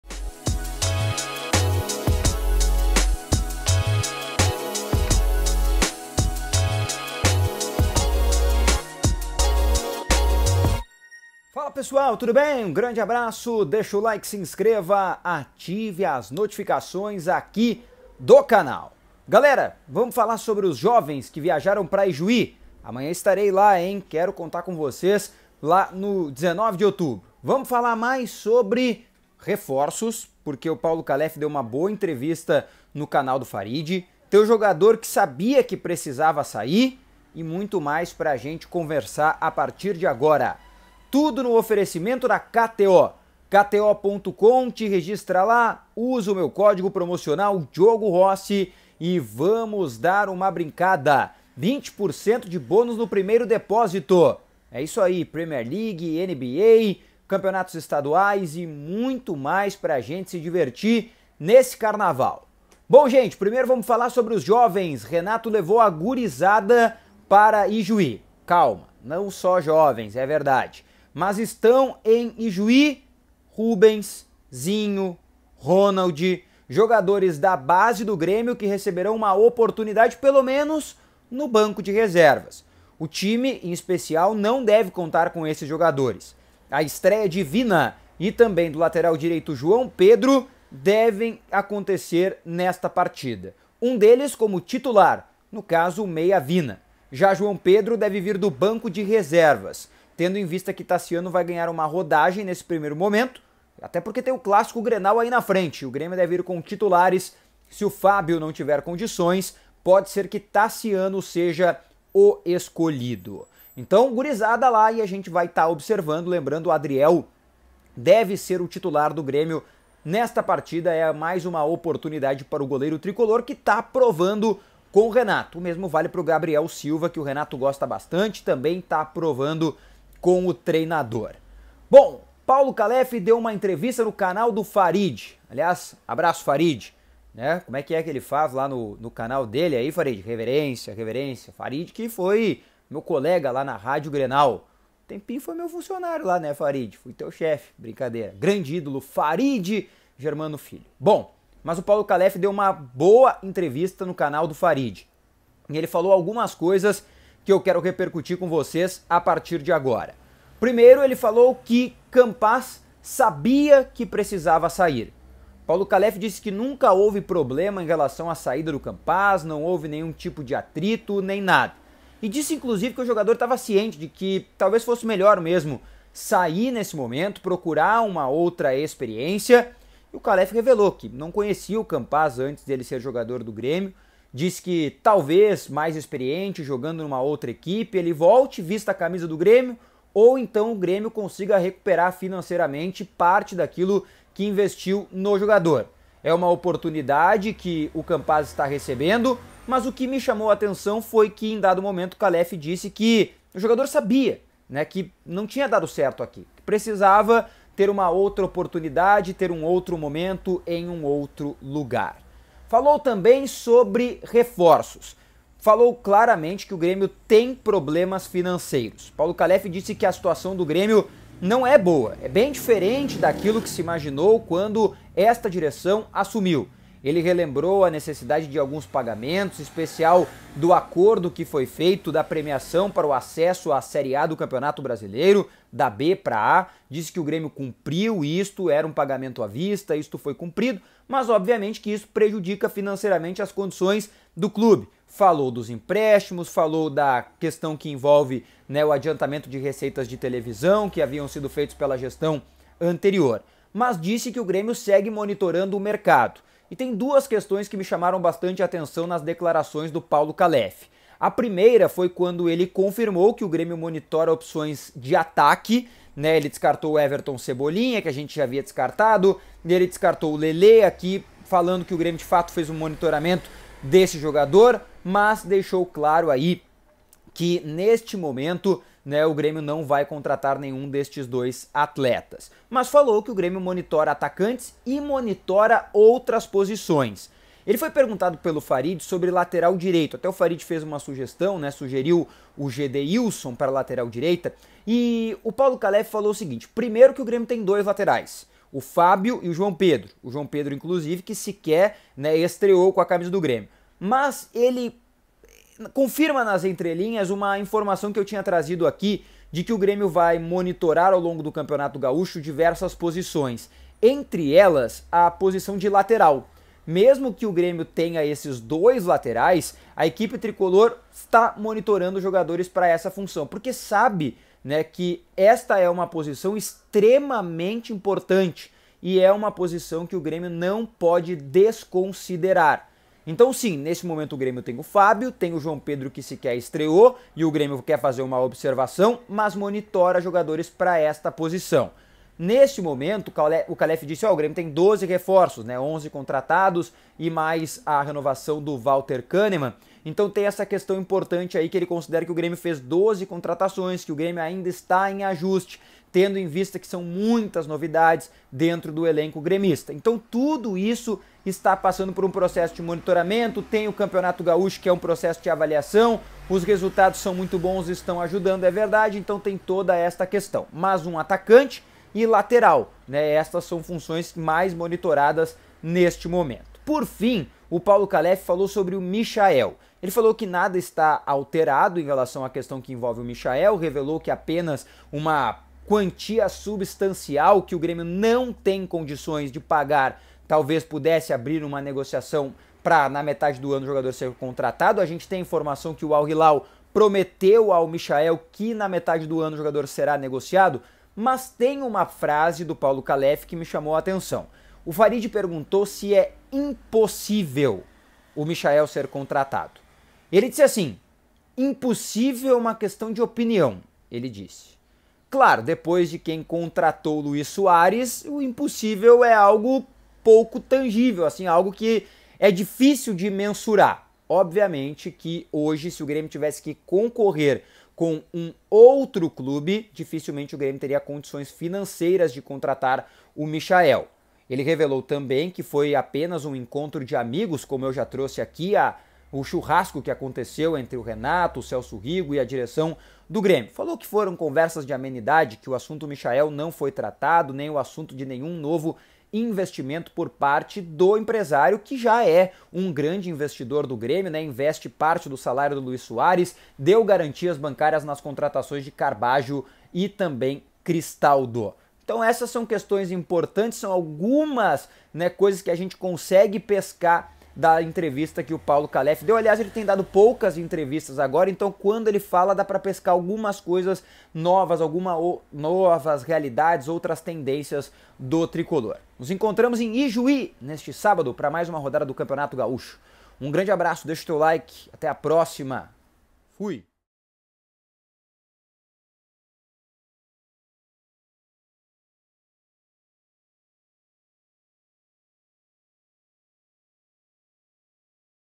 REFORÇOS MONITORADOS | O JOGADOR QUE SABIA QUE PRECISAVA SAIR | MICHAEL E MAIS….
Um resumo da boa entrevista que Paulo Caleffi deu ao @CanaldoFarid.

Reforços? Michael? Quem sabia que precisava sair?

Oferecimento KTO!